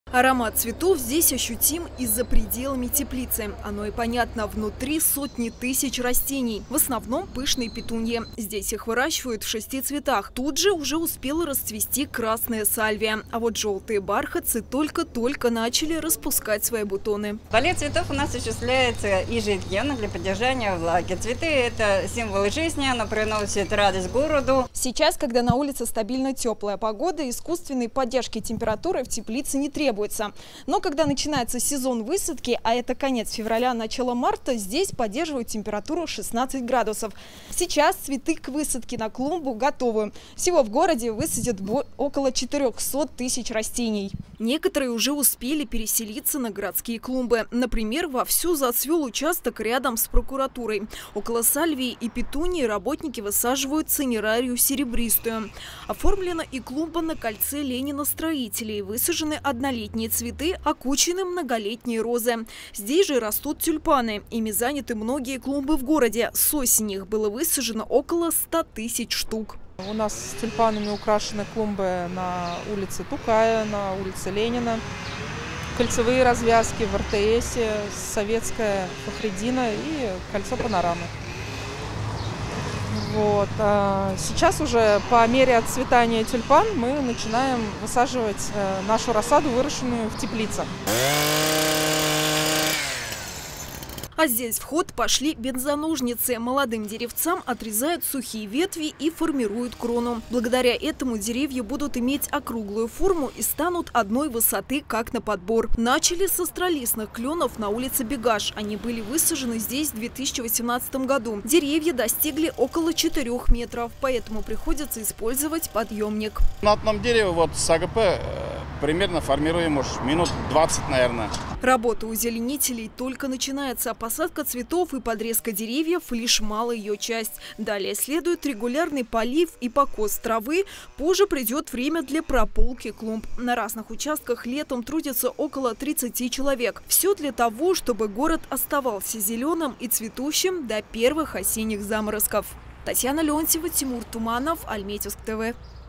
В Альметьевске за сезон высадят до 400 тысяч цветов
В Альметьевске в этом сезоне будет высажено около 400 тысяч цветов. Улицы города уже начали украшать пышными петуниями, сальвиями, бархатцами и другими растениями. Все они были выращены в теплицах.